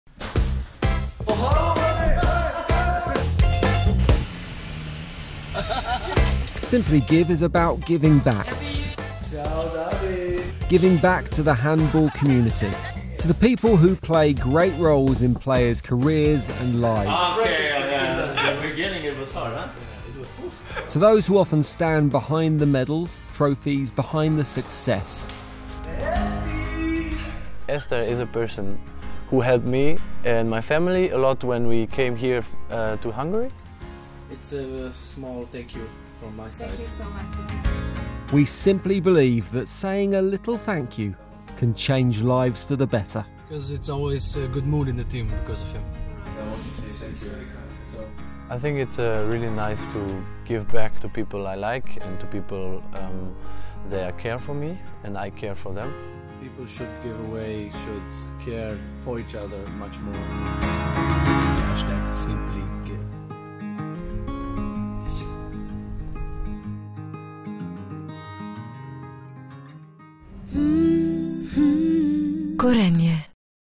SIMPLYGIVE by Gorenje
SIMPLYGIVE and create life-changing moments.

This is the message we want to spread in the upcoming handball season of 2019/20.
SIMPLYGIVE is all about giving as by giving one becomes happier.
We want this to turn into a movement that connects the entire handball community.

We will tell, no even better - we will show you a lot more in the upcoming days, weeks!

P.s.: Did you noticed our brand new monthly ambassador?!